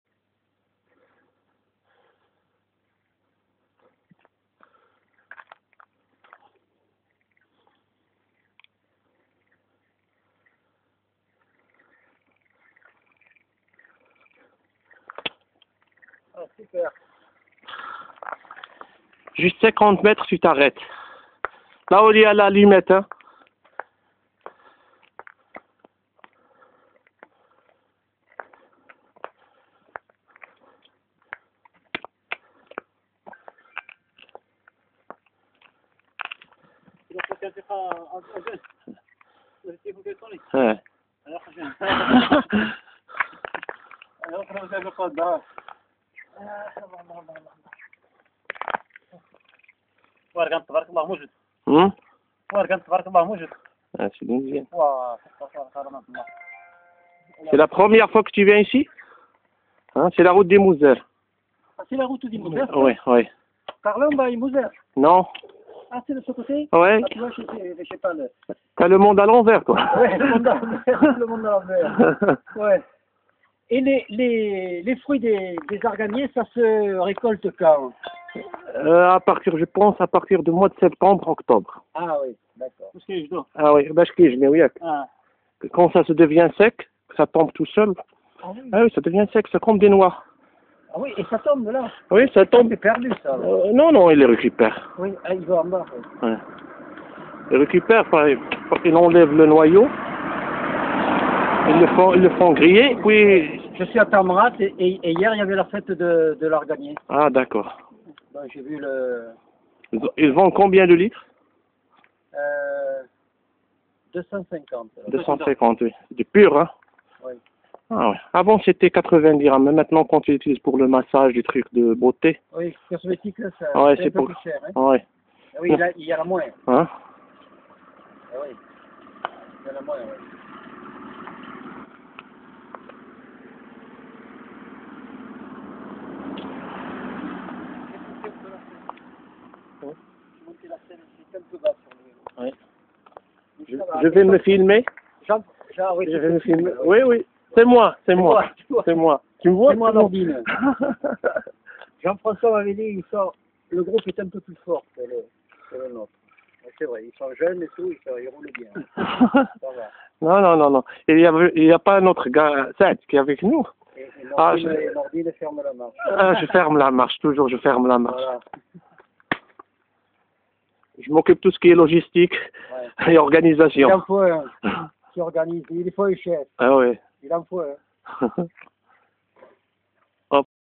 agadirvtt40
Liaison par goudron sur la route d'IMOUZZER pour rejoindre la piste D'imi Mikki